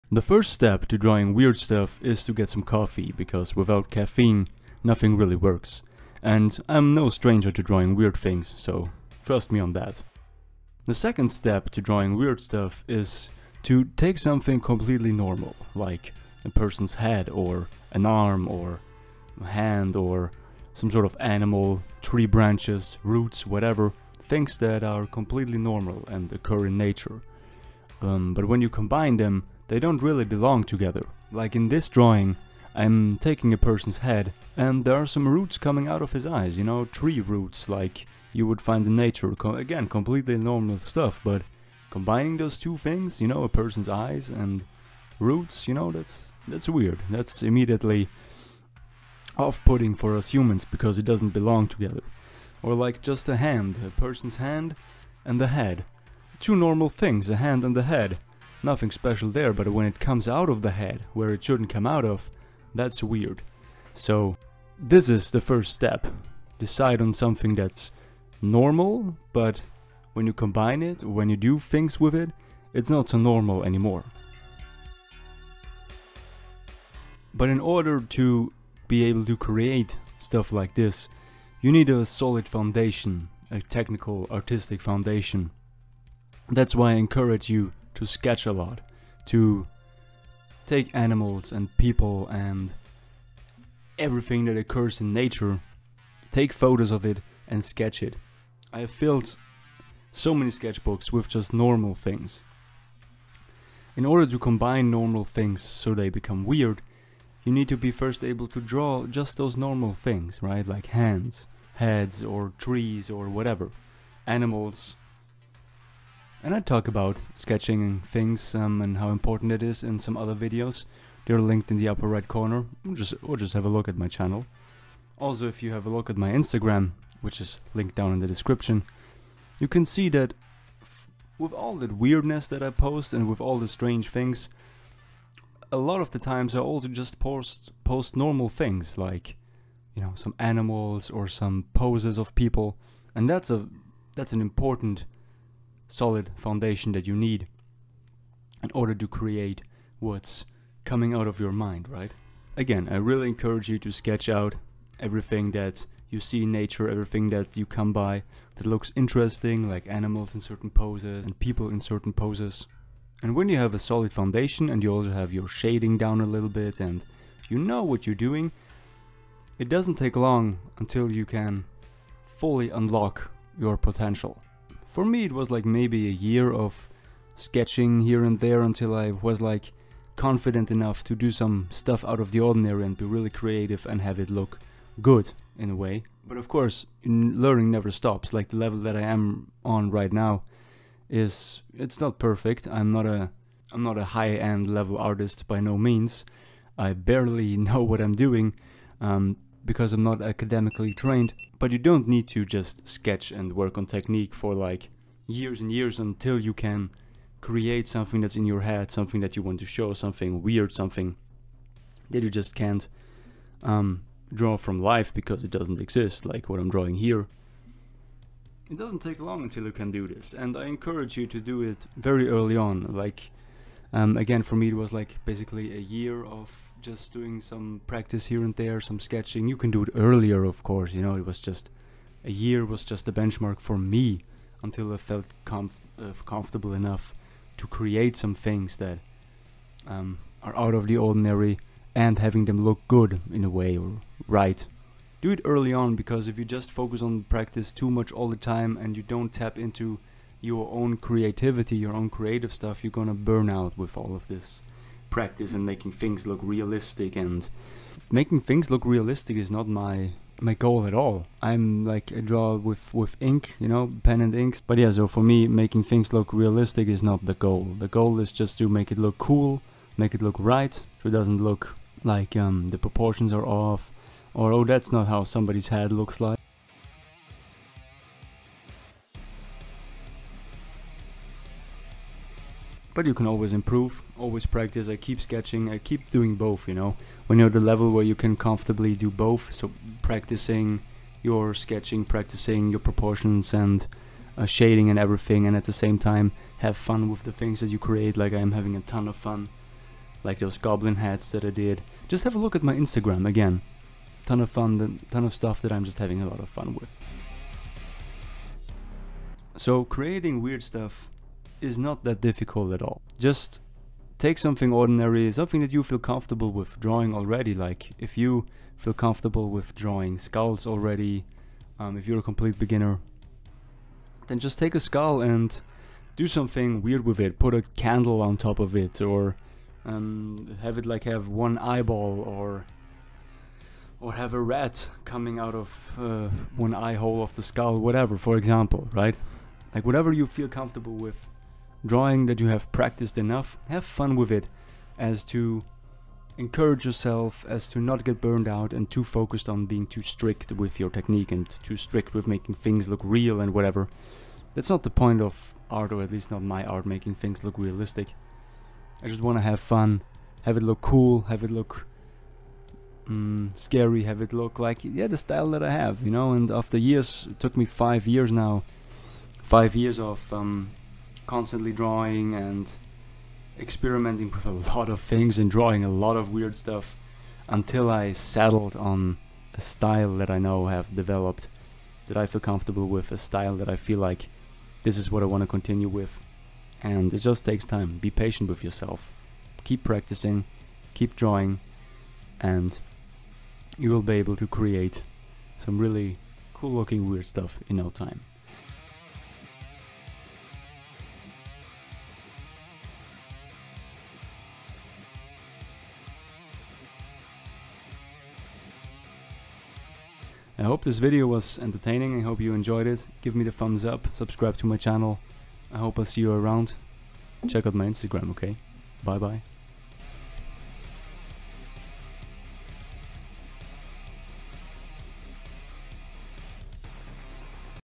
How to Draw Weird Stuff | a Tutorial in Pen and Ink
Simple techniques to improve your creativity and art in general. Tips on how you find your own art style.